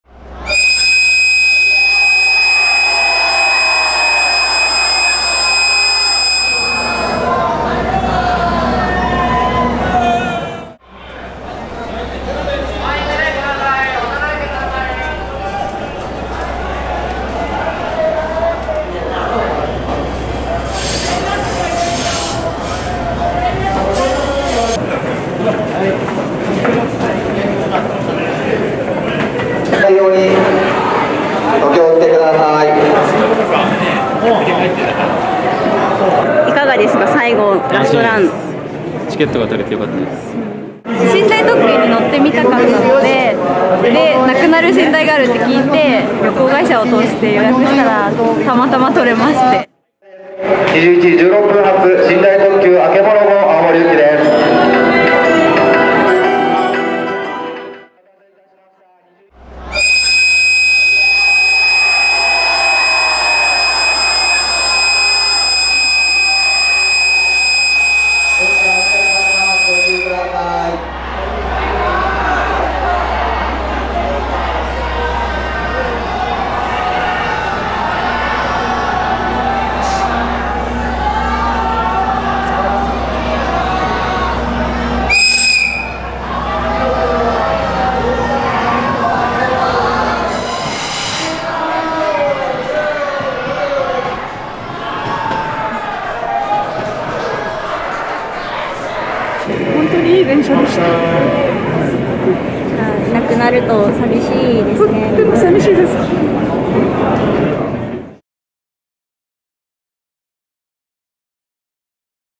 さよなら寝台特急あけぼの ４０年以上の歴史に幕
４０年以上にわたって東京都心と東北を結んだ寝台特急あけぼのが１５日のダイヤ改正で廃止となり、最終列車の雄姿を見届けようと１４日、出発するＪＲ青森、上野両駅に鉄道ファンらが集結した。あけぼのは１９７０年１０月に運行開始。多忙なビジネスマンや旅情を楽しむファンらに親しまれたが、客車が老朽化したほか、新幹線や航空機の利用客が増えて乗車率が約６割と低迷するなどし、廃止が決まった。